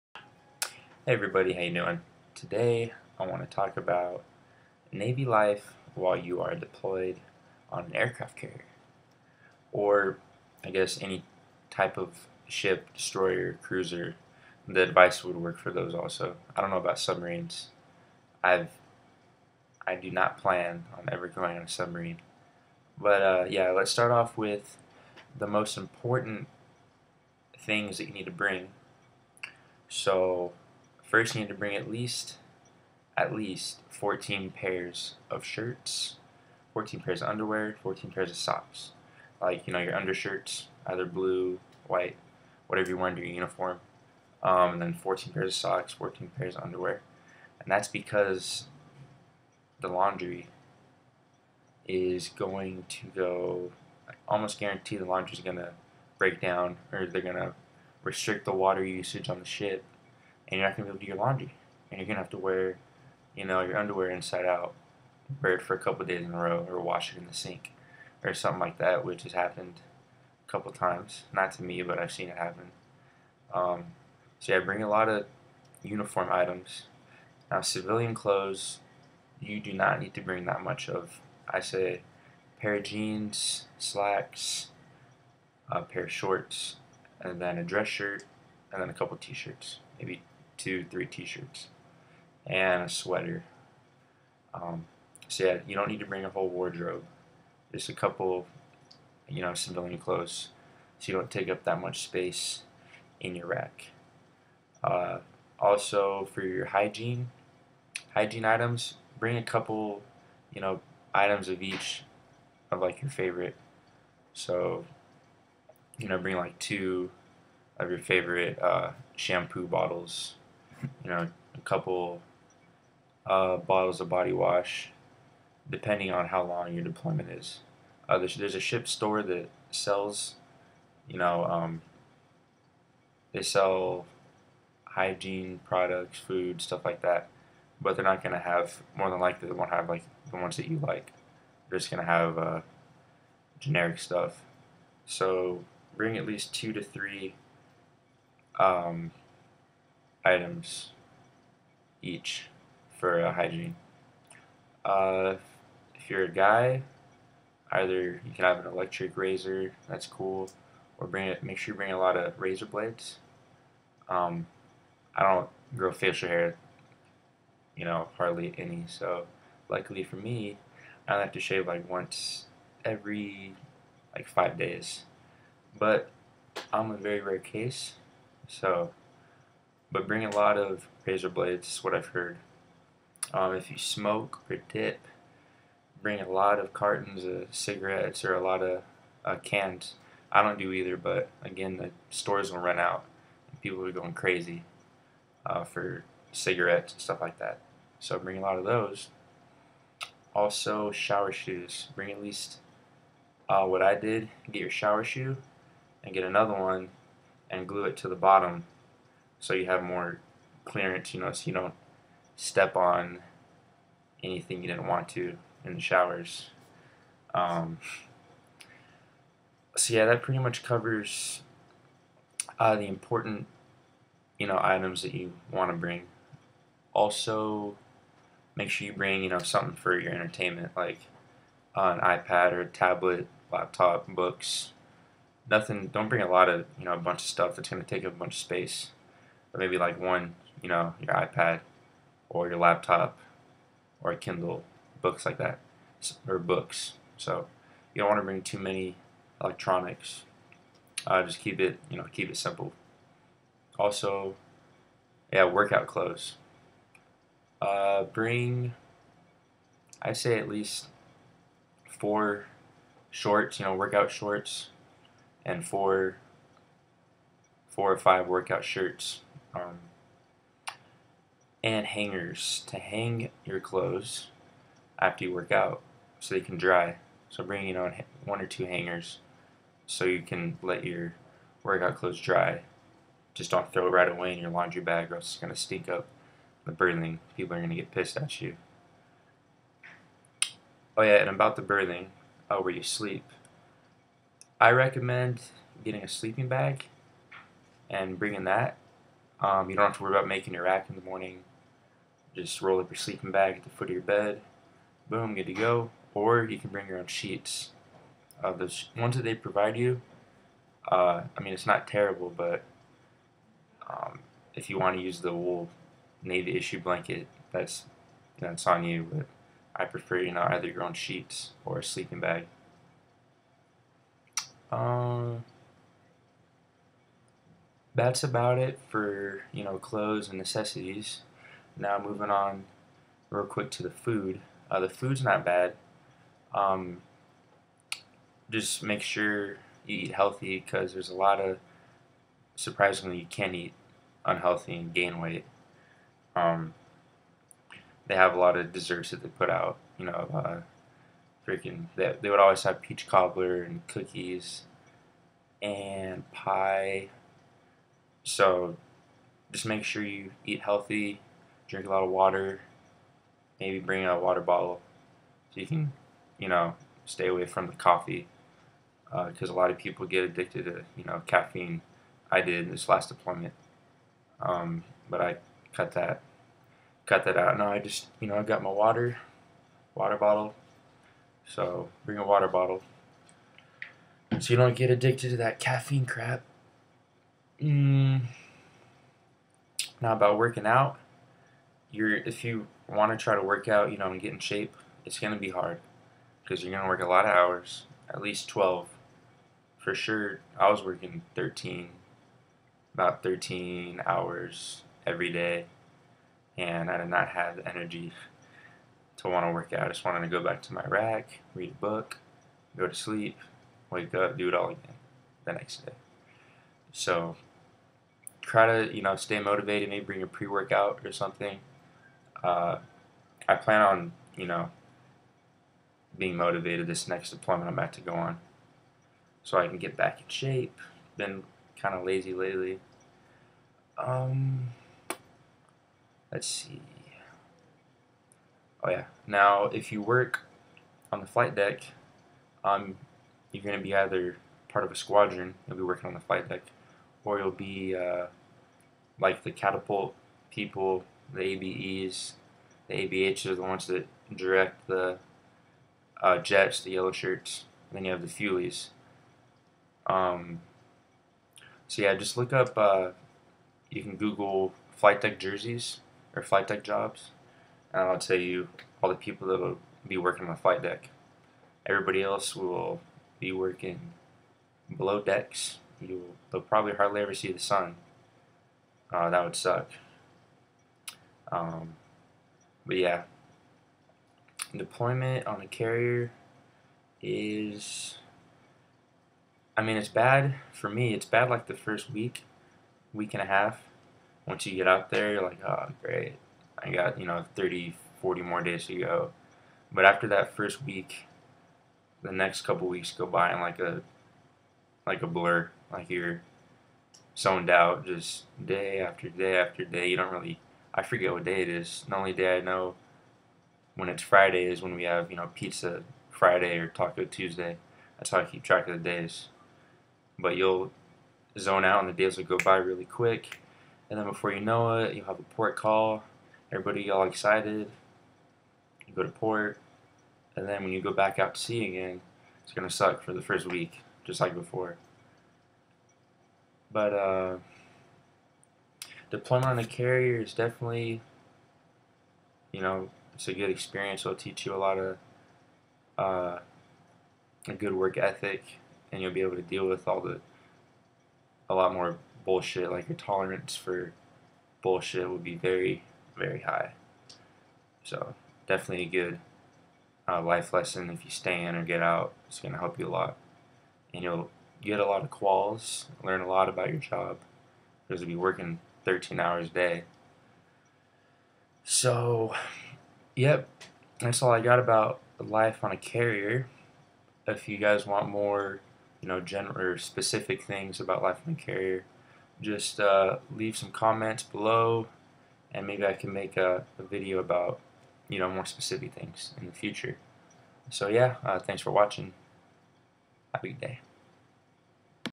Navy Life on a Aircraft Carrier
General Video about Navy life on a carrier.